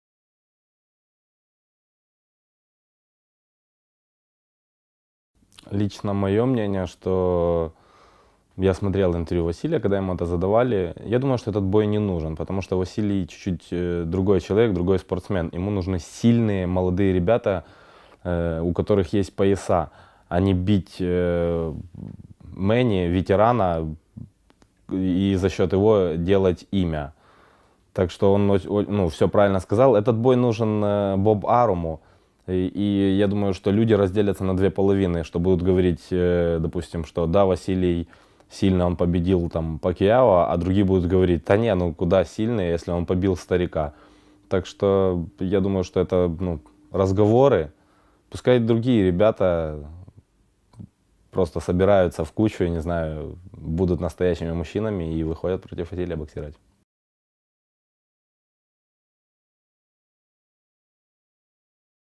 Александр Усик о возможном БОЕ ЛОМАЧЕНКО ПАКЬЯО
АЛЕКСАНДР УСИК О БОЕ ВАСИЛИЯ ЛОМАЧЕНКО С МЭННИ ПАКЬЯО